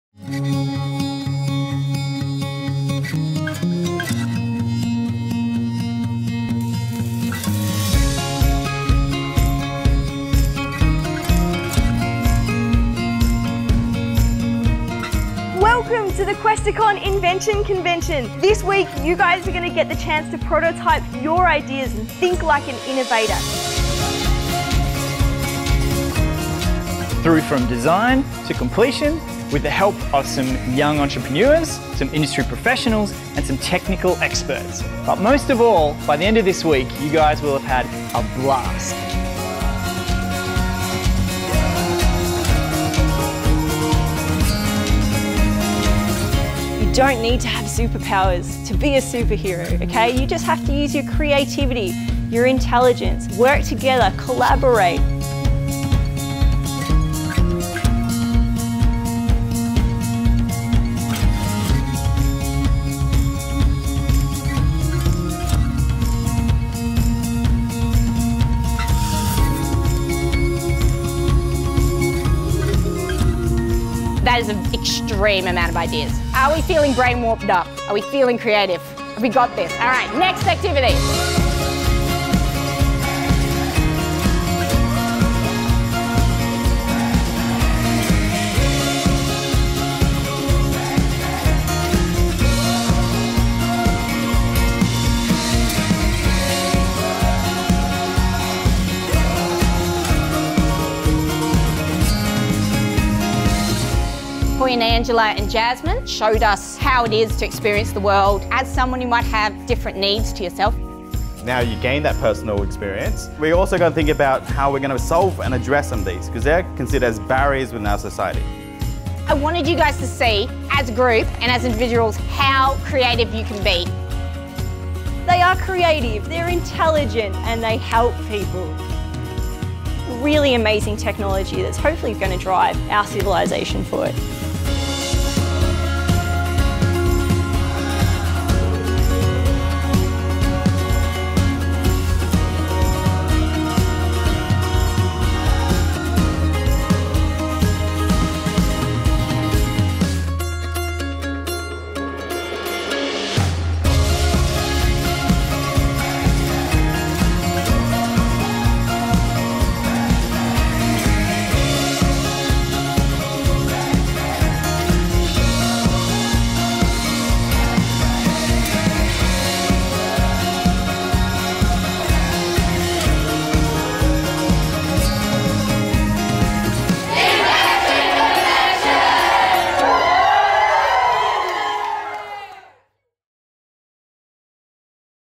Questacon National Invention Convention 2016 Highlights
National and regional workshops where you can work with techsperts from Questacon and innovators, inventors and entrepreneurs from around Australia to turn your ideas into reality.

Who can do this?
14-18 year olds attending an Australian secondary school

When and where can you get involved?

2016 dates

18-22 January - The national Questacon Invention Convention
The Ian Potter Foundation Technology Learning Centre, Canberra

19-21 July - Questacon Invention Convention
Whyalla and Adelaide (Mawson Lakes), SA

27-29 September - Questacon Invention Convention
Gold Coast, QLD